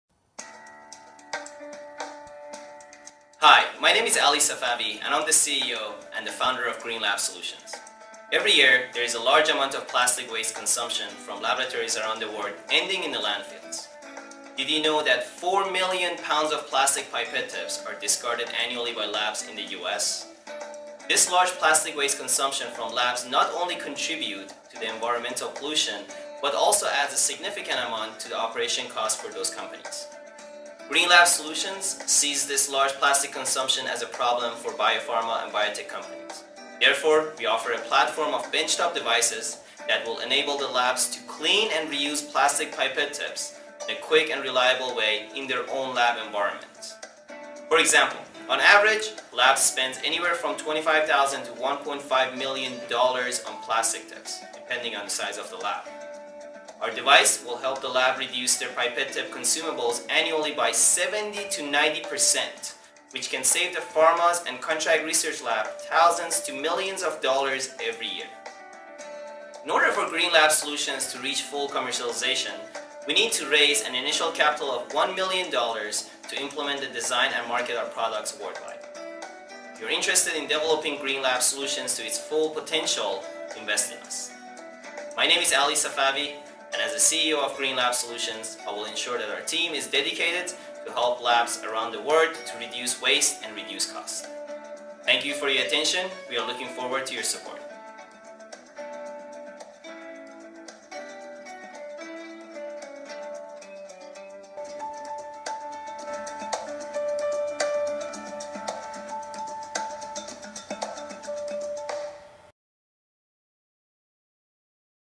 Green Lab Solutions Elevator Speech
Elevator Speech of Green Lab Solutions.

Green Lab Solutions is a start-up launched by four students from Duke University, NC.

Green Lab Solutions offers innovative, high-quality products to generate cost-benefits for customers by reducing plastic consumable waste in the laboratory environment.